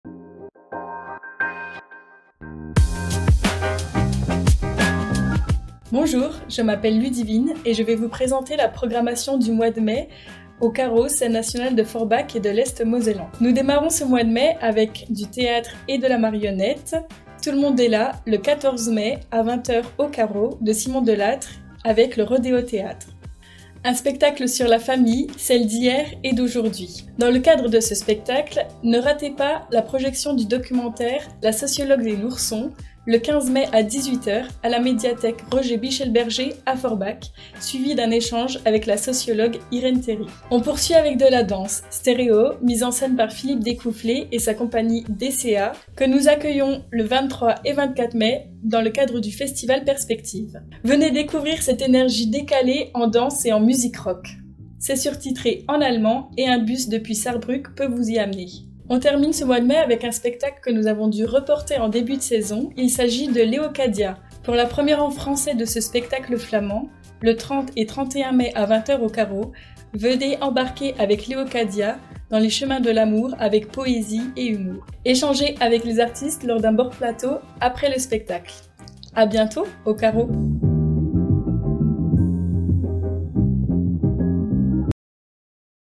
Agenda Mai 2024 du Carreau
Comme chaque mois, le carreau présent son programme de Mai 2024 en vidéo. En partenariat avec FOR'FM.